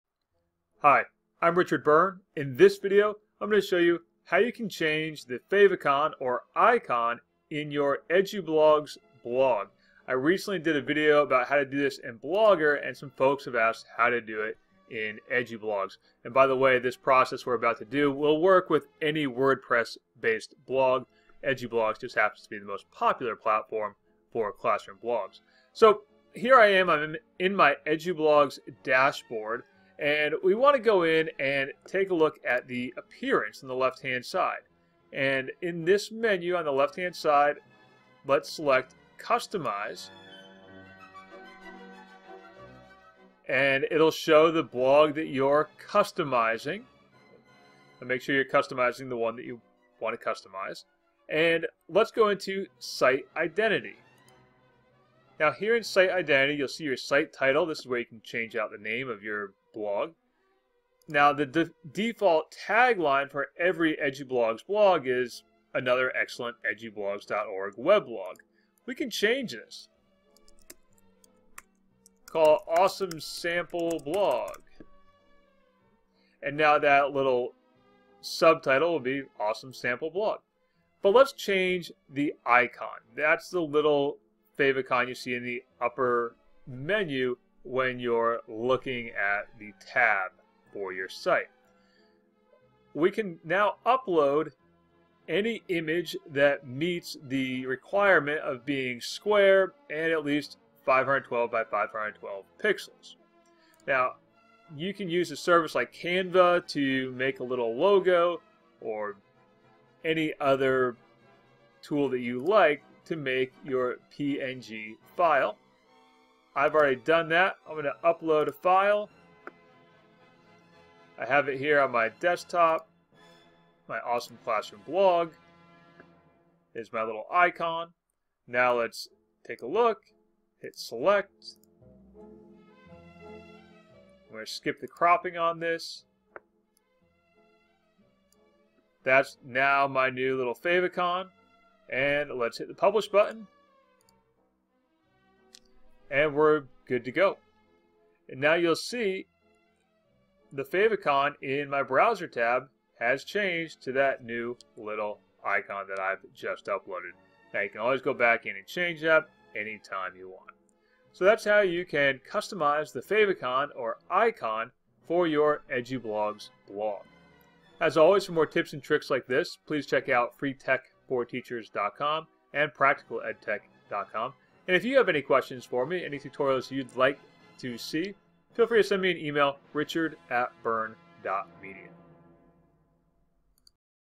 How to Change the Favicon for Your Edublogs Blog
You can change the favicon of your Edublogs blog or any other Wordpress-based blog by using this simple method.